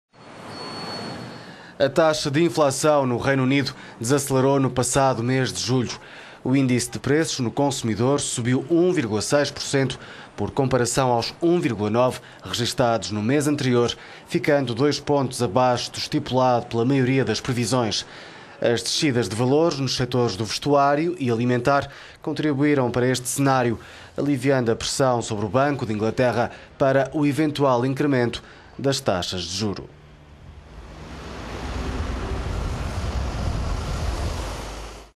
Inflação britânica abranda no início do 2o. semestre - economy
A taxa de inflação no Reino Unido desacelerou no passado mês de julho. O índice de preços no consumidor subiu 1,6%, por comparação aos 1,9% registados no mês anterior, ficando dois pontos abaixo do estipulado pela maioria das previsões. As descidas de valores nos setores do vestuário e alimentar contribuíram para este cenário, aliviando a pressão sobre o Banco de Inglaterra para o eventual incremento das taxas de juro.…